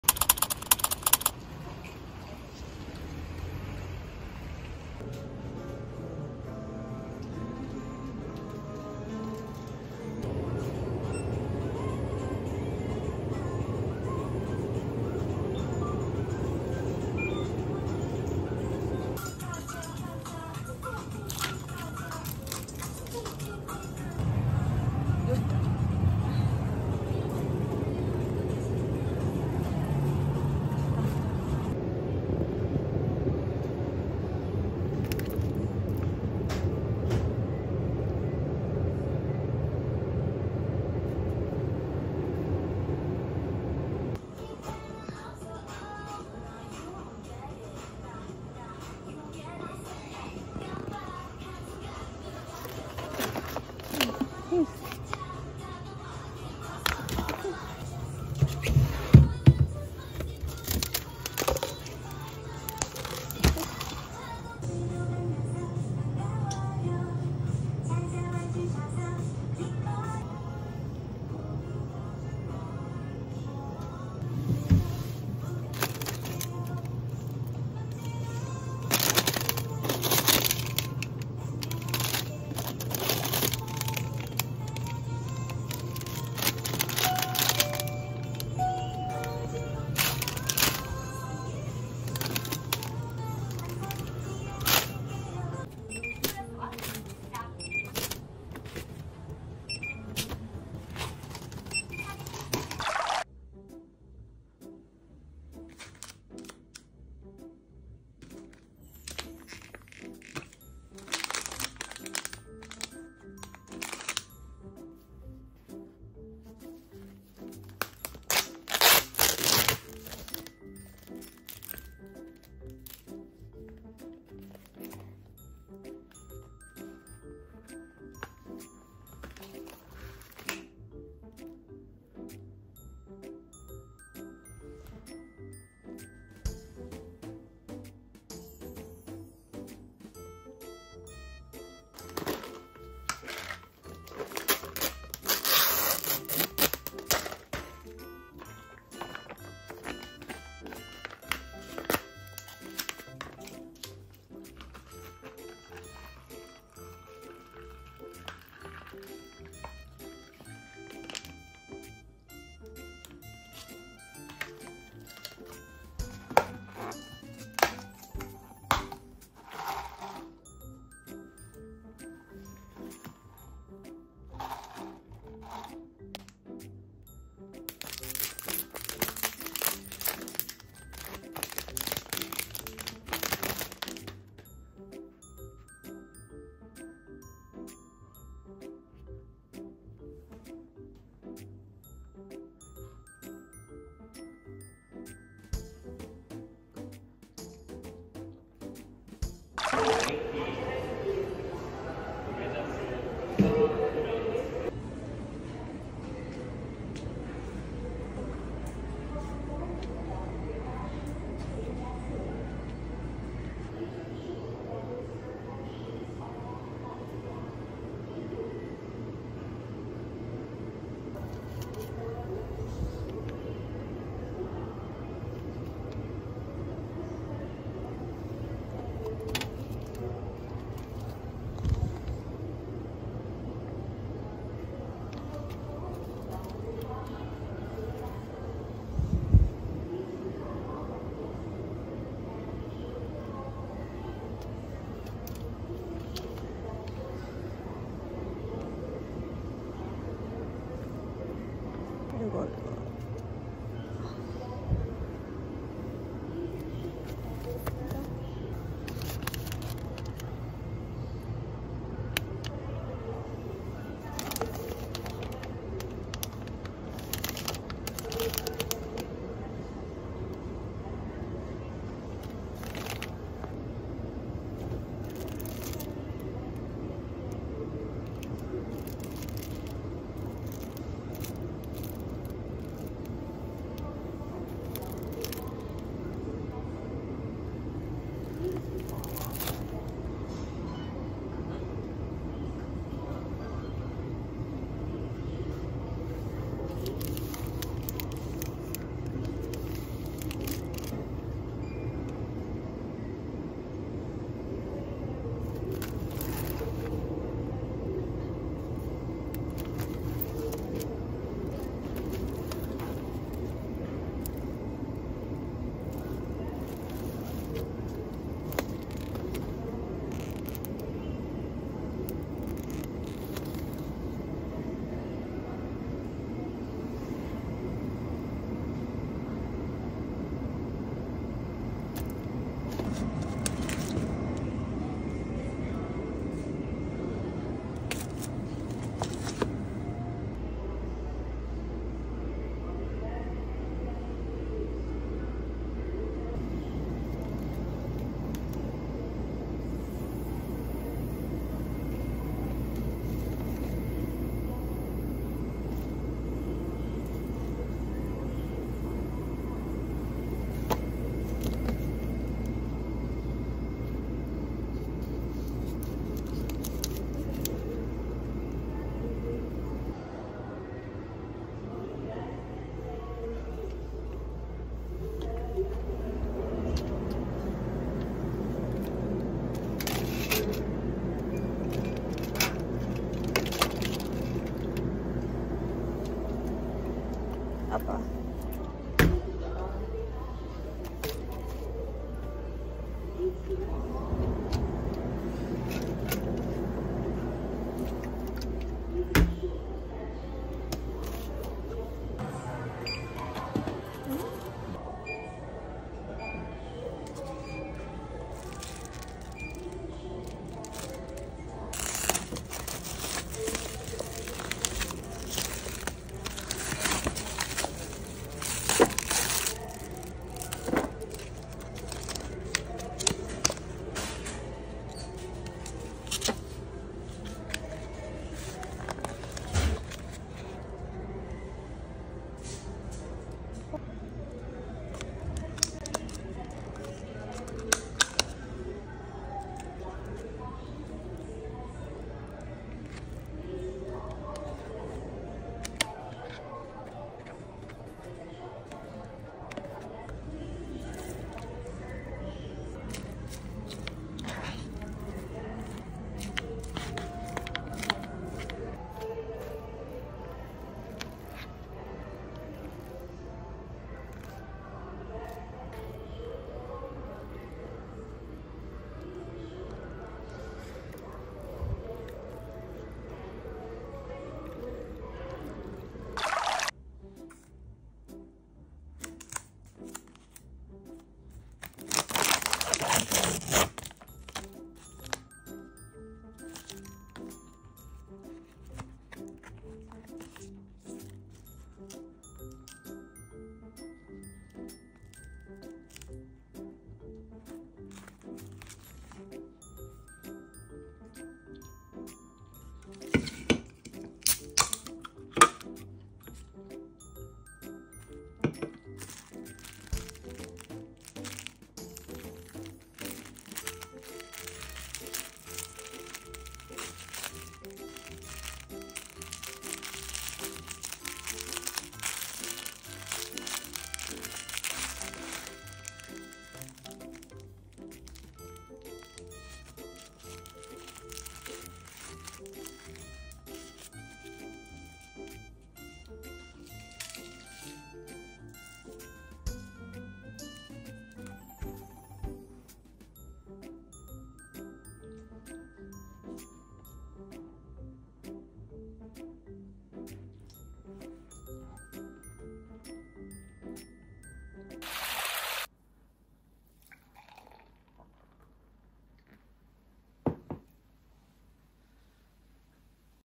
shopping in korea vlog 🇰🇷 convenience store food 🍱 lunch boxes, sandwich, coffee & desserts
Korean convenient store food challenge. Three meals a day! 🍱 check out food they sell at Korean CVS with prices. Wide variety of items such as lunch boxes, sandwiches, coffee & desserts. 🧋🍰